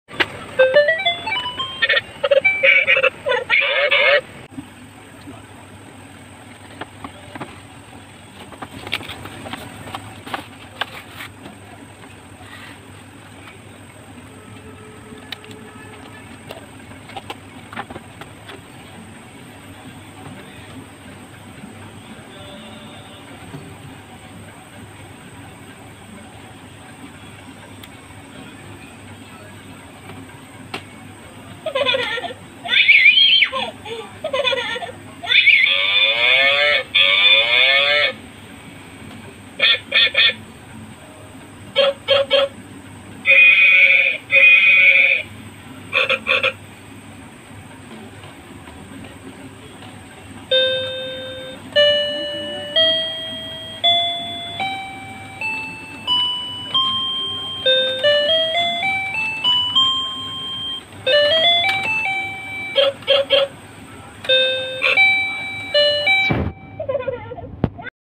Little Rabbit Piano 🐸, 🐑, 🐕, 🤗, 🐄, 🦆 It's Fun Come on and try it 1080p
Little Rabbit Piano. It's Fun. Come on and try it. music Ubiquity, Give baby a Happy childhood Time
Switch Key: - Off/On
MODE TWO
8KEY: 8Music
🐸 Frog Key:- Croak+rhythm
🐑 Sheep Key:- Bleat+rhythm
🐕 Dog Key:- Bark+rhythm
🤗 Child Key:- child Call+ rhythm
🐄 Cow Key:- moo+rhythm
🦆 quack+rhythm
MODE ONE
Frog: Croak
Sheep: Bleat
Dog: Bark
Child: child Call
Cow: Moo
Duck: quack